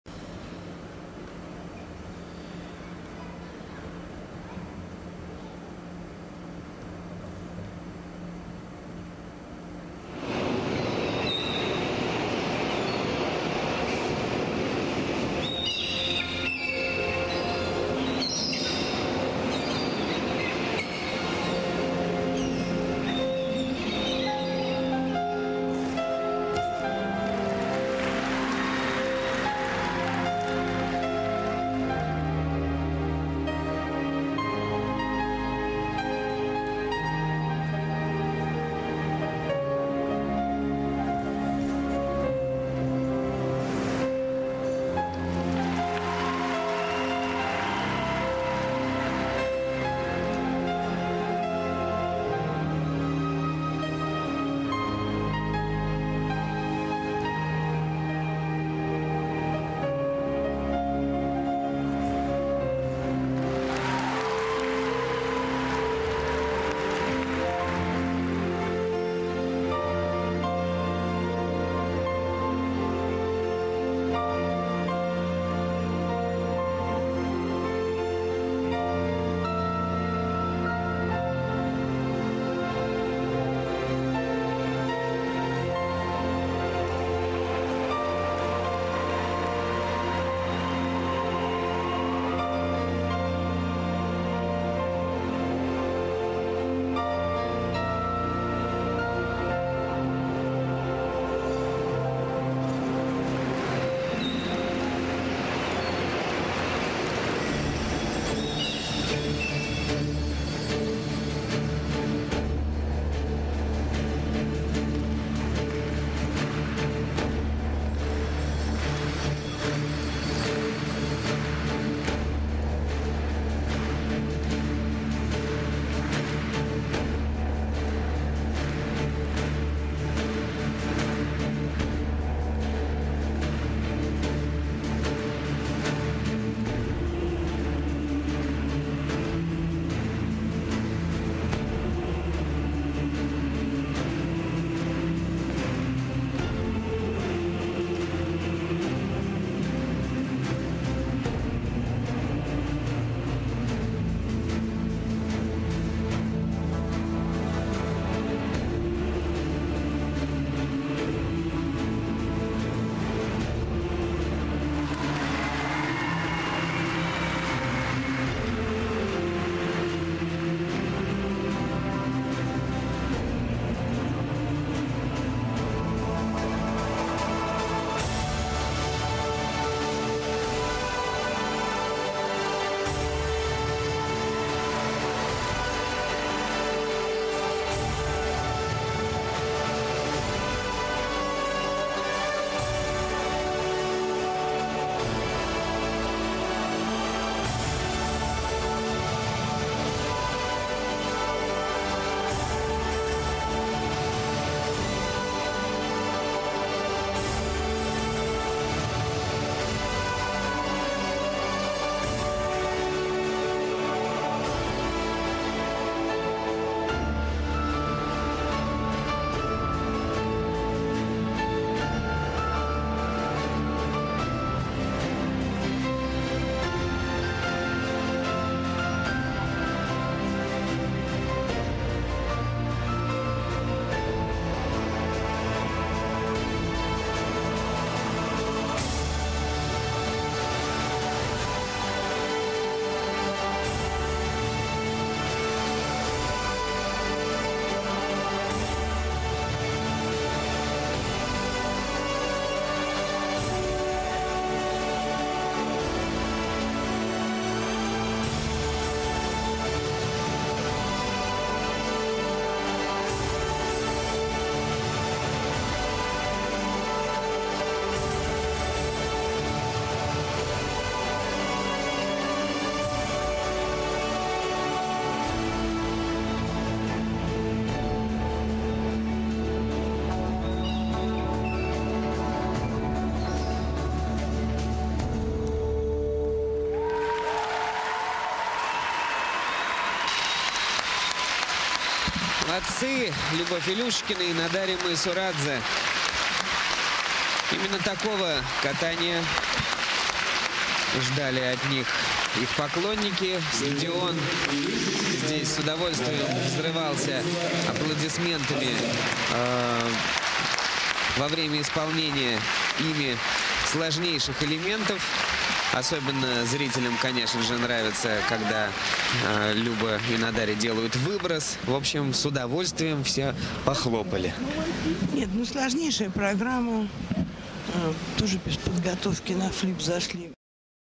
Lubov ILIUSHECHKINA Nodari MAISURADZE 2011 LP Russian Nationals
Произвольная программа, Чемпионат России 2011, Саранск
(4th/5th, 120,81/179,49pts, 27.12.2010) Любовь Илюшечкина / Нодари Майсурадзе